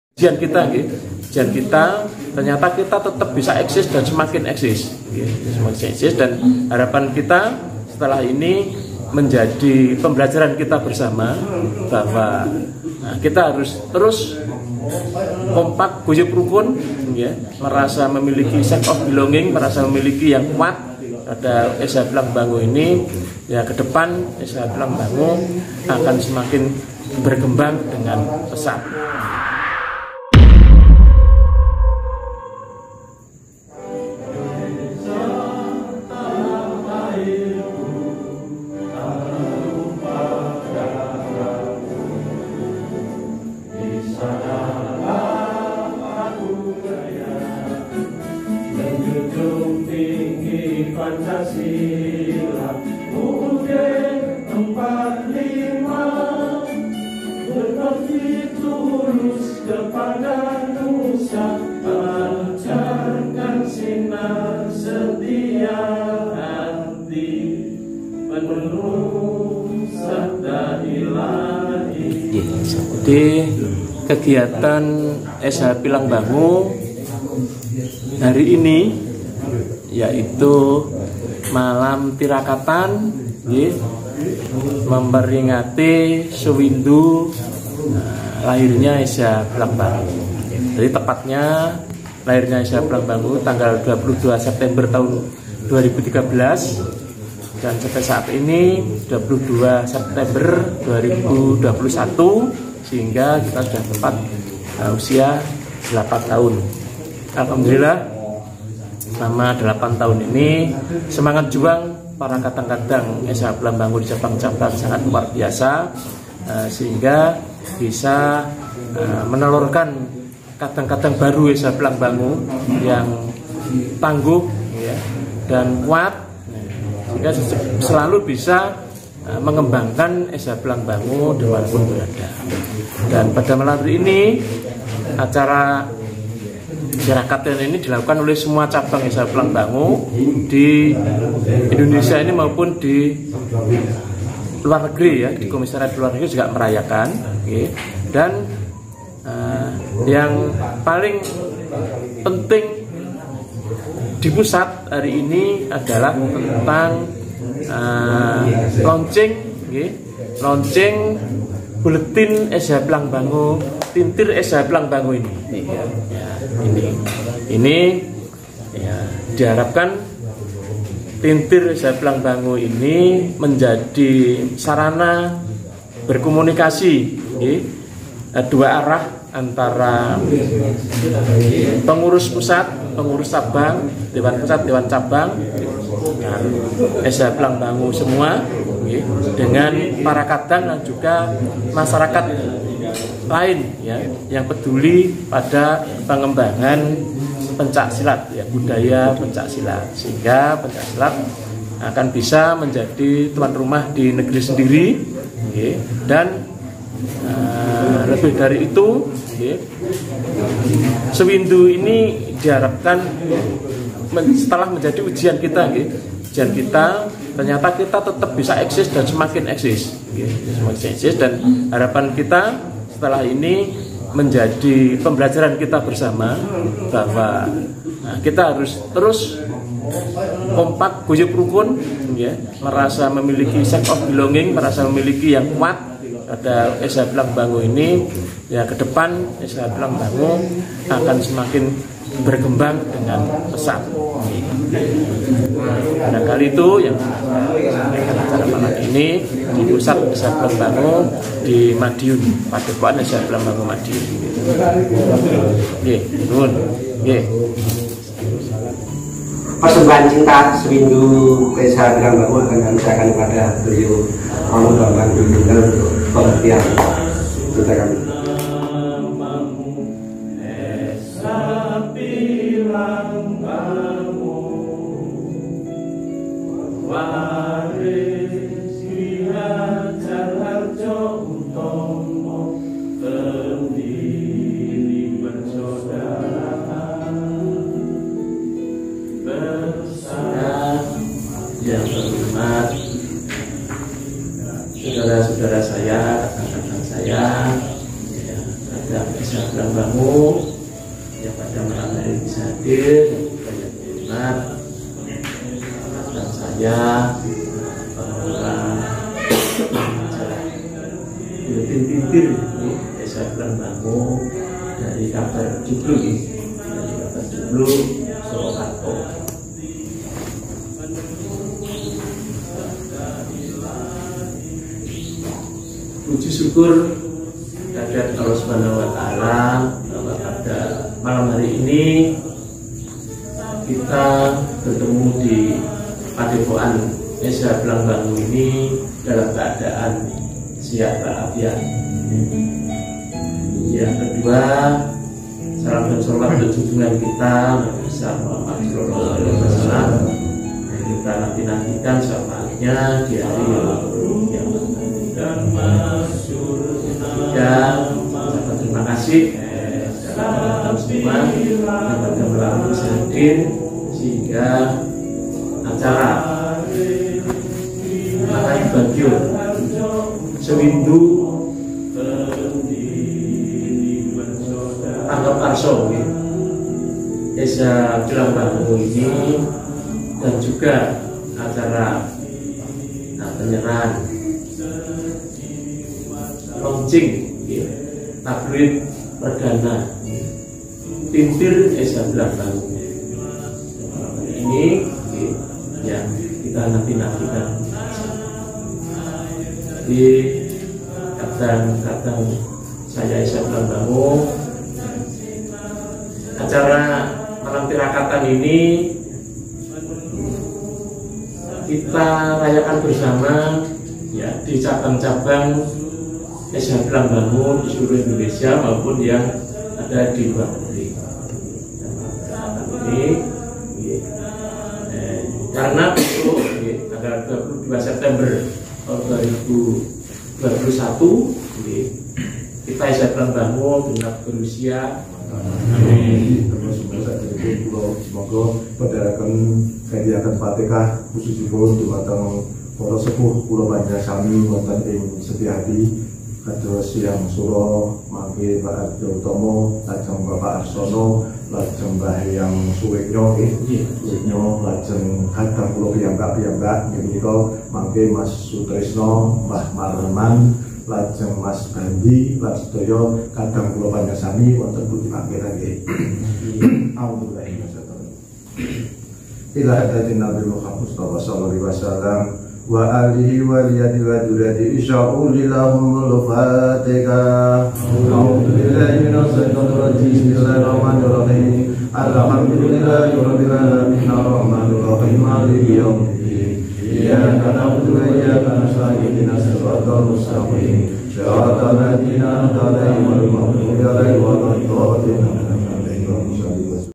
Tirakatan Wilujeng Sewindu PSH Pilangbango sekaligus Launching Majalah Thinthier eSHa
Tirakatan Wilujeng Sewindu PSH Pilangbango sekaligus Launching Majalah Thinthier eSHa di Padepokan Pusat SH pilangbango Madiun.Rabu(21/9)malam